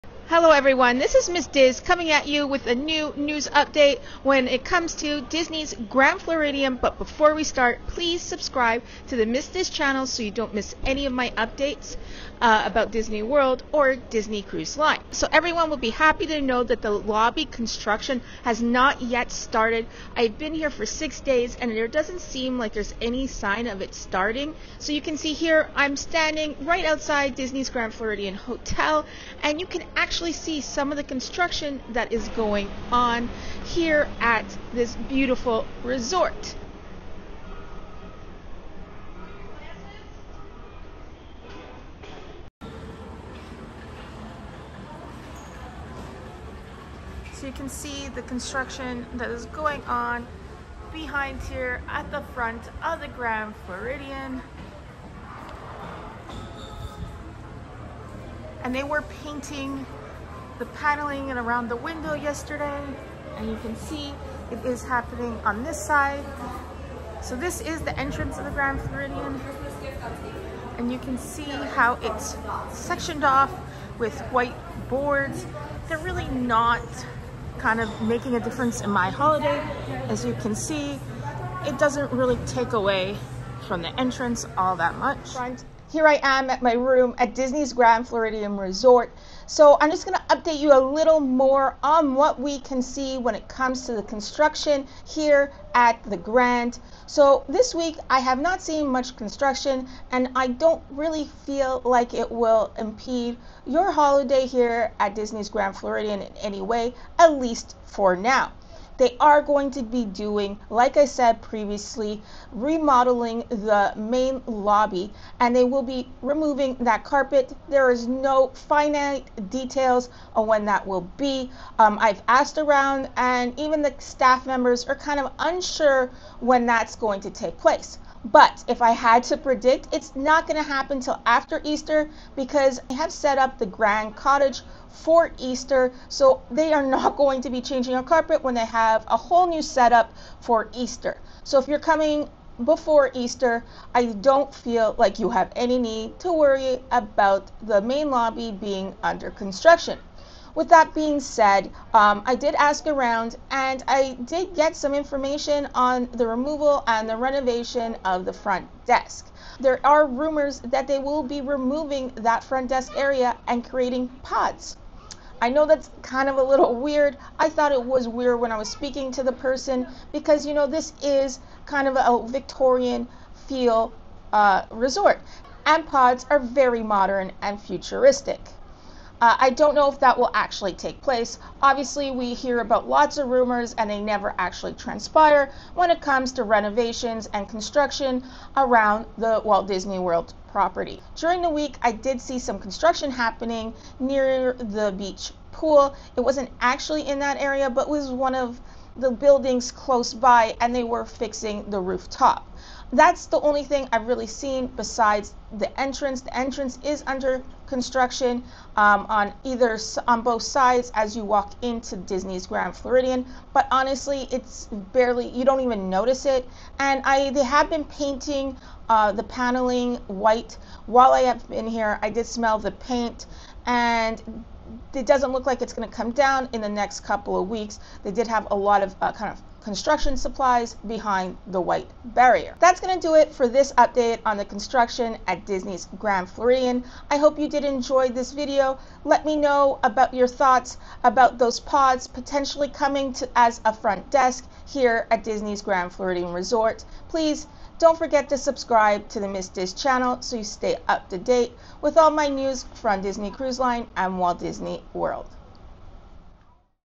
Disney's Grand Floridian Resort Refurbishment Update - FRONT DESK Being Turned into PODS?
In this vlog, I take you live to Disney's Grand Floridian Resort & Spa for the latest refurbishment update. Most of the construction is happening at the entrance and at the moment the lobby is not being updated. I also heard a rumor that the front desk is being turned into pods!